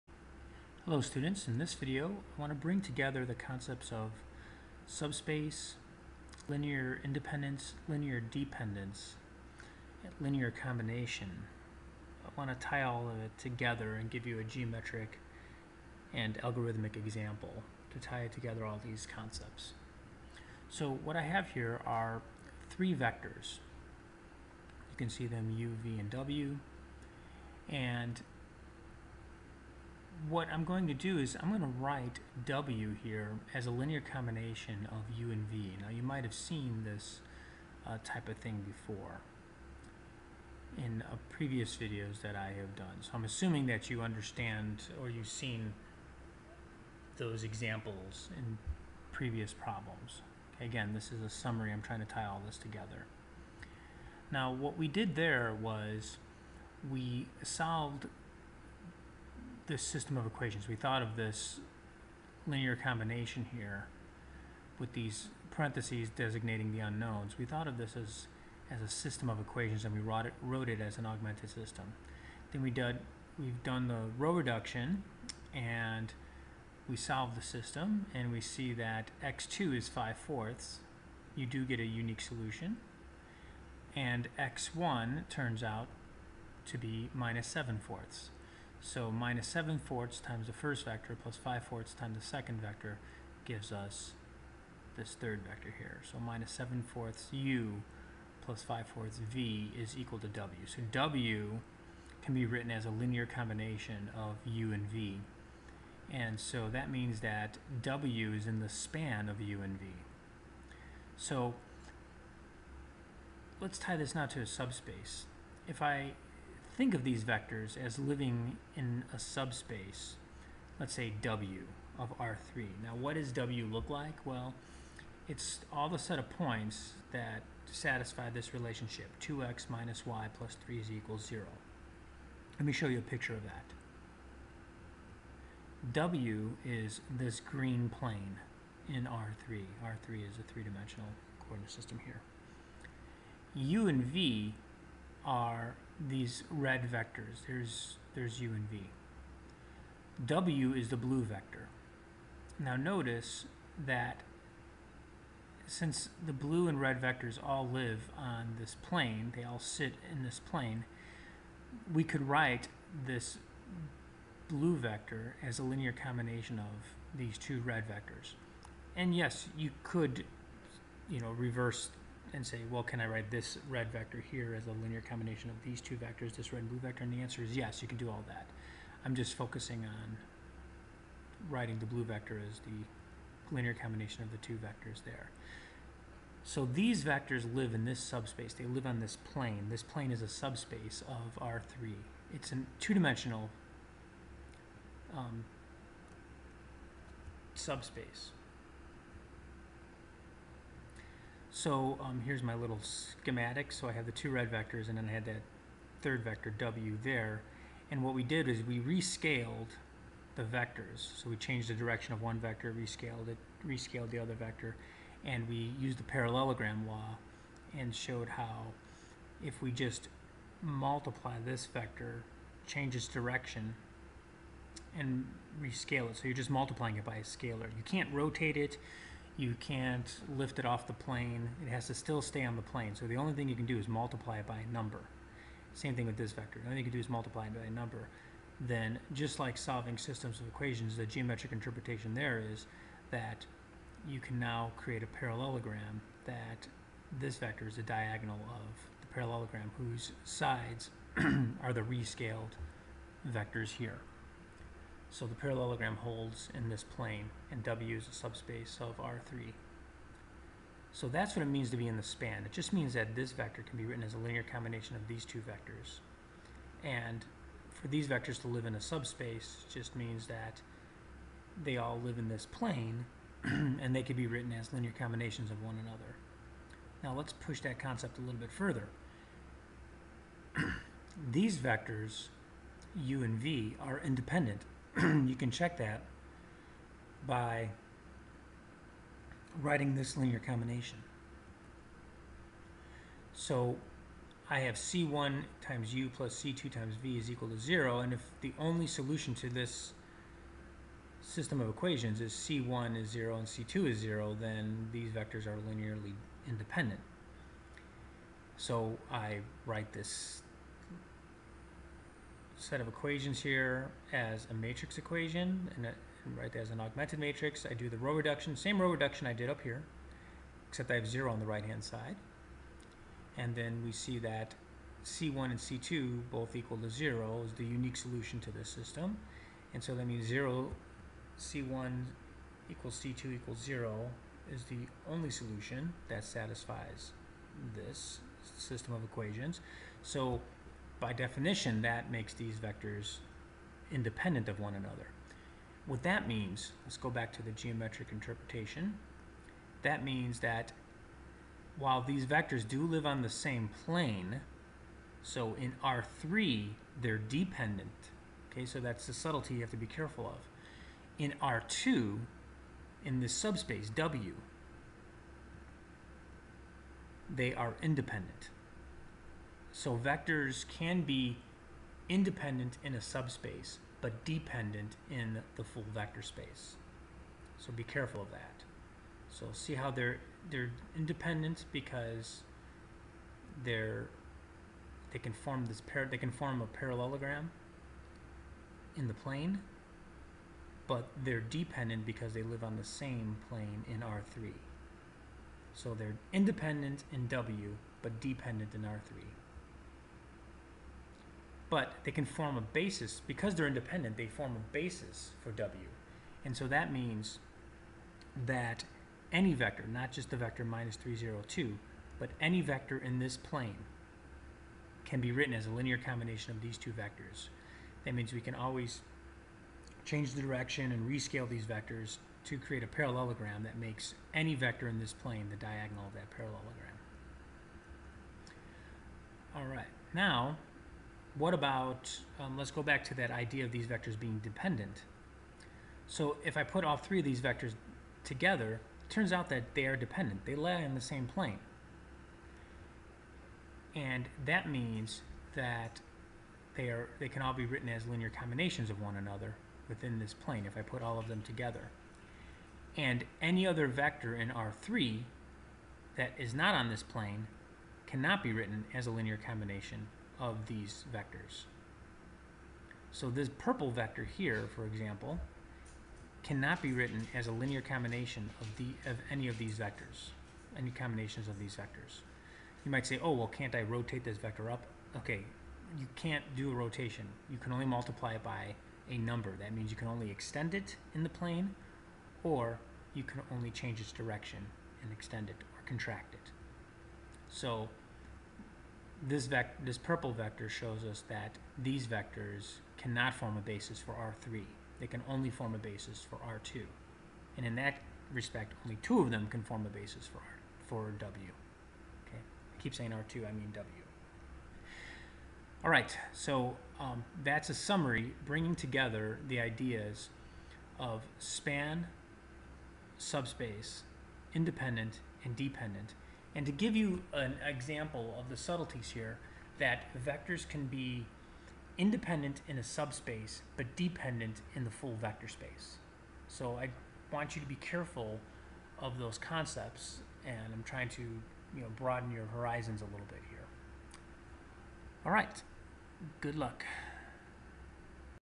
Summary of Subspace, Span, and Basis
We bring together the ideas of subspaces, independence, dependence, span, and basis using a single example. One of the subtleties is that vectors can be independent in a subspace but dependent in the full vector space.